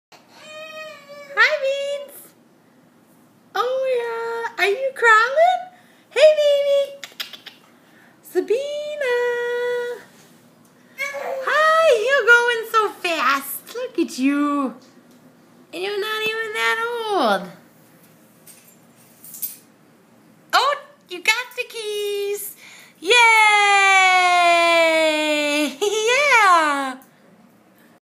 The Beanster crawling!
Sabina is crawling at 8 months